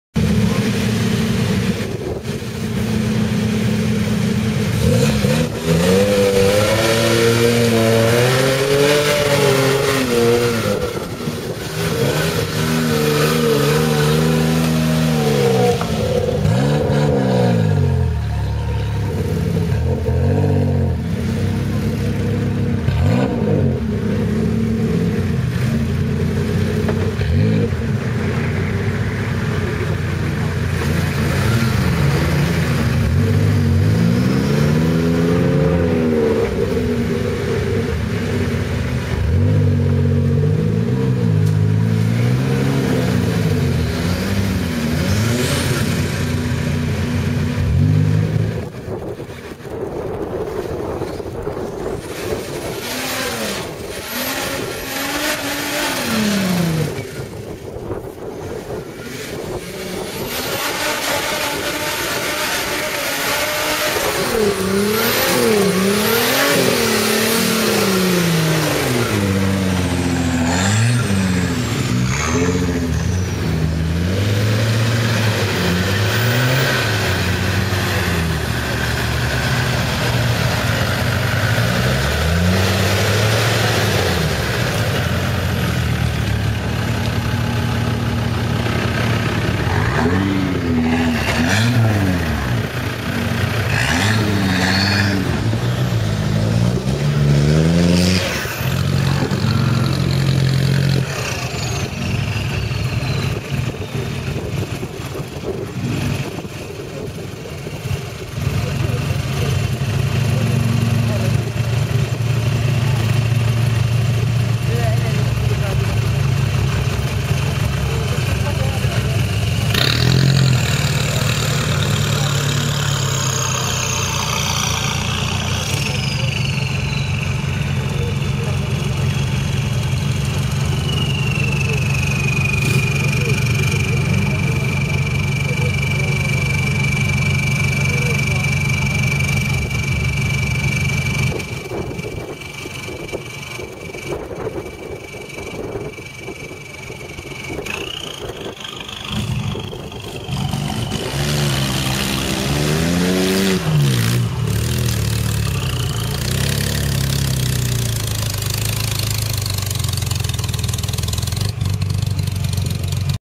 Transmadiranaise 2013
montée faites par un proto, deux jeep, un land et un toy